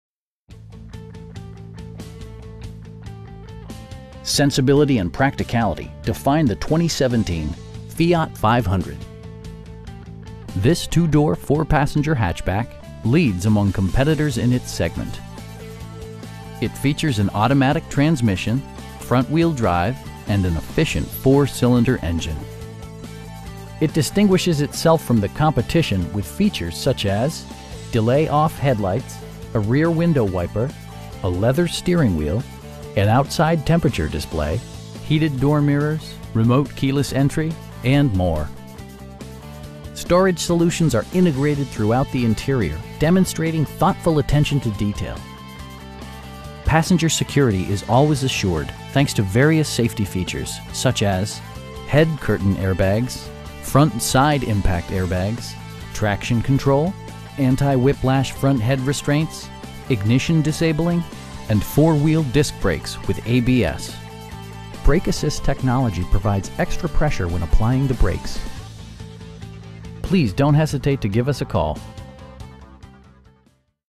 2017 FIAT 500 Pop in Milford, NH 03055
Contemporary Chrysler Dodge Jeep Ram

Sensibility and practicality define the 2017 Fiat 500. This 2 door, 4 passenger hatchback leads among competitors in its segment. It features an automatic transmission, front-wheel drive, and an efficient 4 cylinder engine. It distinguishes itself from the competition with features such as: delay-off headlights, a rear window wiper, a leather steering wheel, an outside temperature display, heated door mirrors, remote keyless entry, and more. Storage solutions are integrated throughout the interior, demonstrating thoughtful attention to detail. Passenger security is always assured thanks to various safety features, such as: head curtain airbags, front side impact airbags, traction control, anti-whiplash front head restraints, ignition disabling, and 4 wheel disc brakes with ABS. Brake assist technology provides extra pressure when applying the brakes. We have the vehicle you've been searching for at a price you can afford. Please don't hesitate to give us a call.